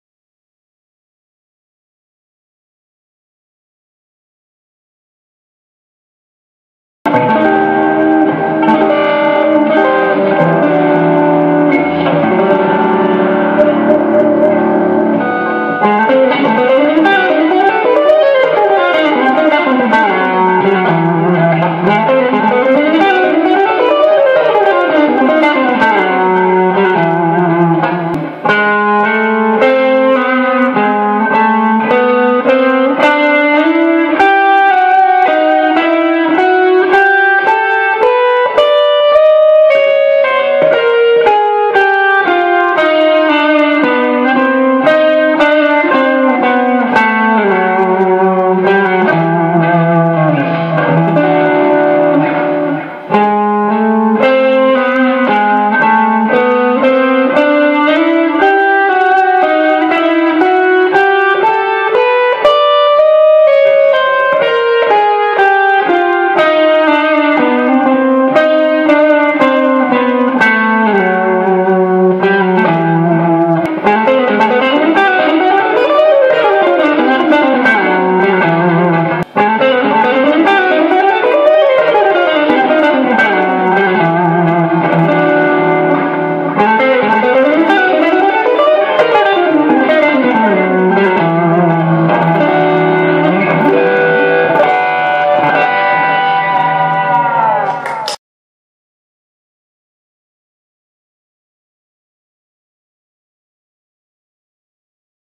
Lick 105 - Frase out side. André Victor !
Fraseado Fusion gravado com CS3, SD- 1, Reverb do ampli, Echo delay e minha velha Fender Richie Sambora.